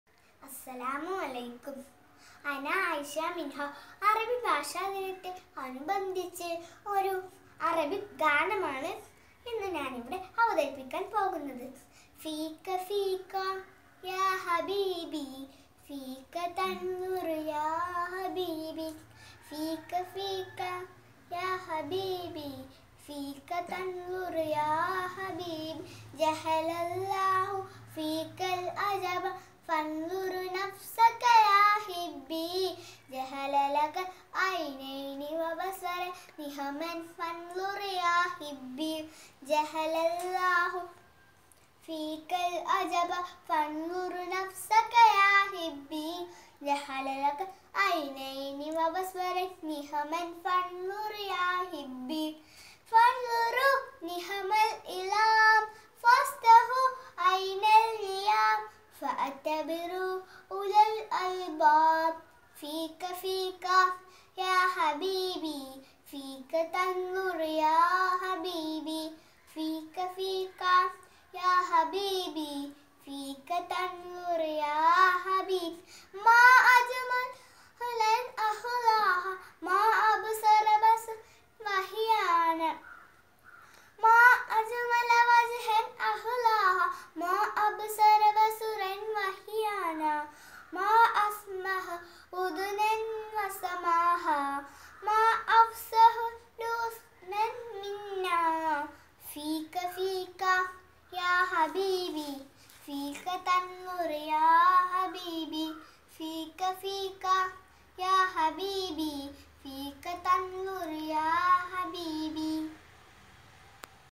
അറബി ഗാനവുമായി ഒന്നാം ക്ലാസുക്കാരി ആയിശാ മിൻഹ
അന്താരാഷ്ട്ര അറബി ഭാഷാ ദിനത്തോടനുബന്ധിച്ച് പരന്നേക്കാട് ജെ.എം.എൽപി.സ്ക്കൂൾ അറബിക്ക് ക്ലബിന്റെ കീഴിൽ വിദ്യാർത്ഥികൾക്കായി സംഘടിപ്പിച്ച CHILDREN'S PROGRAMME - അറബിഗാനവുമായി ആയിശാ മിൻഹ (1C)